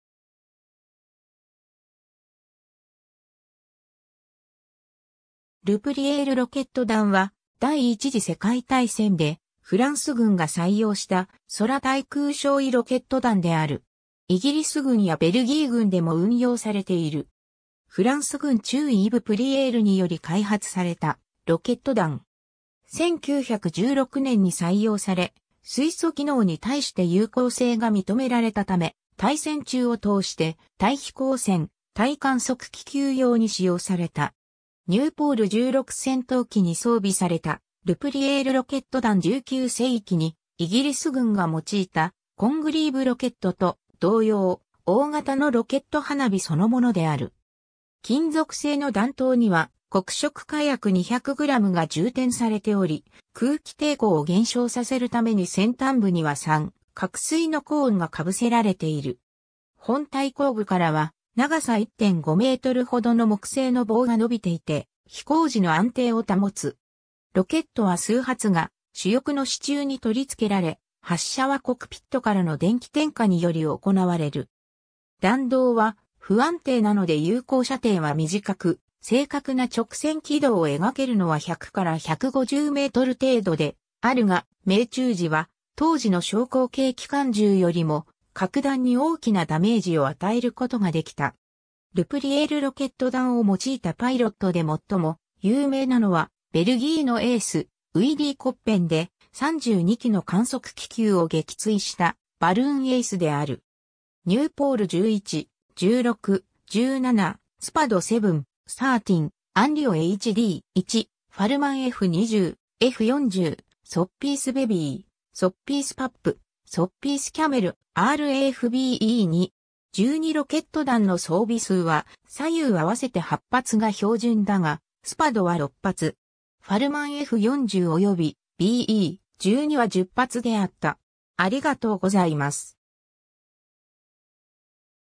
ル・プリエールロケット
ル・プリエールロケット弾 （Le Prieur rocket）は第一次世界大戦でフランス軍が採用した空対空焼夷ロケット弾である。
イギリス軍やベルギー軍でも運用されている。
 フランス軍中尉イヴ・プリエール（Yves le Prieur）により開発されたロケット弾。
1916年に採用され、水素気嚢に対して有効性が認められたため、大戦中を通して対飛行船、対観測気球用に使用された。
 ニューポール16戦闘機に装備されたル・プリエールロケット弾 19世紀にイギリス軍が用いた「コングリーヴ・ロケット」と同様、大型のロケット花火その物である。
 金属製の弾頭には黒色火薬200gが充填されており、空気抵抗を減少させるために先端部には三角錐のコーンが被せられている。
本体後部からは長さ1.5m程の木製（松）の棒が伸びていて飛行時の安定を保つ。
 ロケットは数発が主翼の支柱に取り付けられ、発射はコクピットからの電気点火により行われる。
弾道は不安定なので有効射程は短く、正確な直線軌道を描けるのは100から150m程度（115m説が有力）であるが、命中時は当時の小口径機関銃よりも格段に大きなダメージを与える事が出来た。
 ル・プリエールロケット弾を用いたパイロットで最も有名なのはベルギーのエース、ウイリー・コッペン（Willy Coppens）で、32基の観測気球を撃墜した「バルーンエース」である。
 ニューポール 11/16/17、SPAD VII/ XIII、アンリオ HD.1、ファルマンF.20/F.40、ソッピース ベビー、ソッピース パップ、ソッピース キャメル、RAF B.E.2/.12 ロケット弾の装備数は左右合わせて8発が標準だが、SPADは6発。
ファルマンF.40及びBE.12は10発であった。